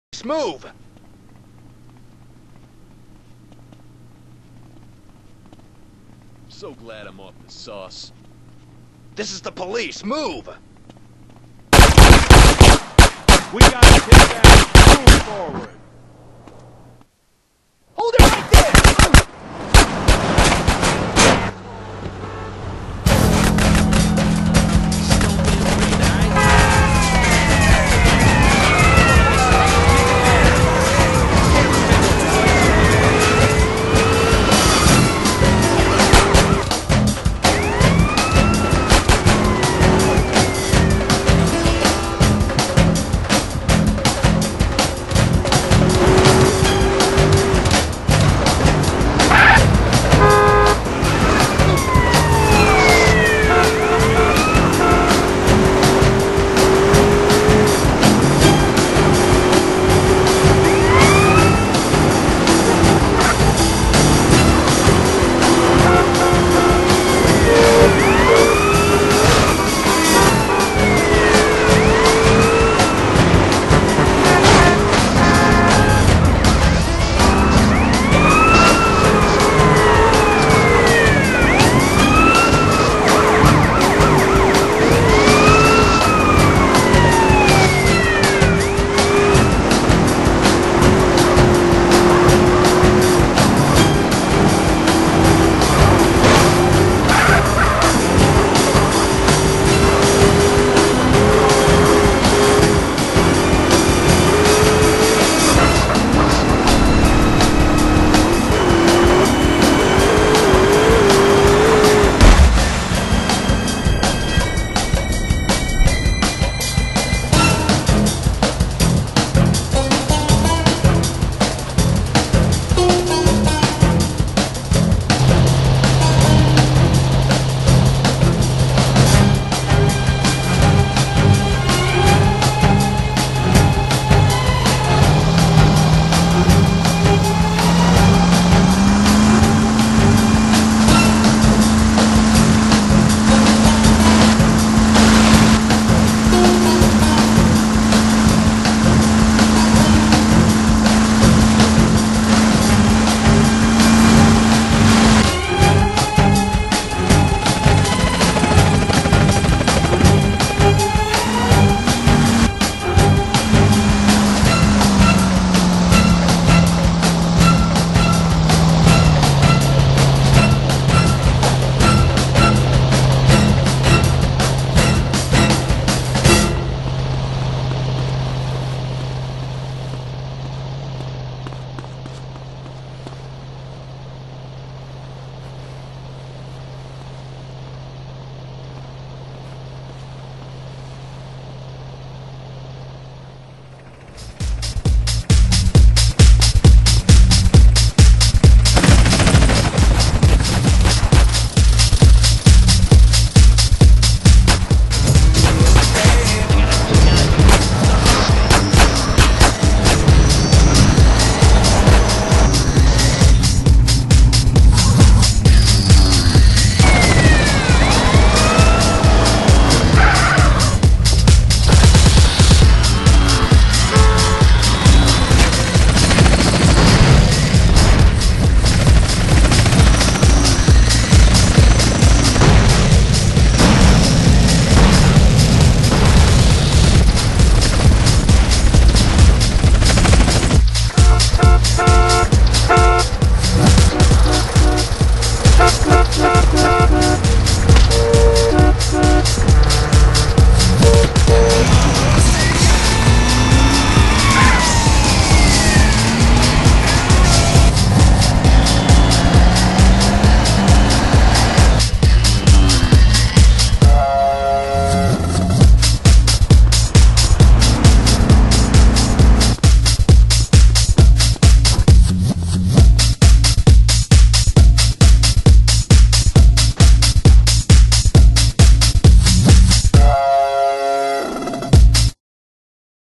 The Assasins Episode 5
James and jones arrived in LSPD
but they like law breaking
so cops chased them
can they escape?

The Assasins (C)Phantompunch Entertaiment